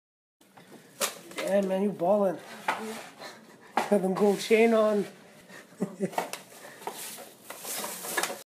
BEAGLE WEARING 14k Gold Chain BLING BLING!!
Beagle wears a gold chain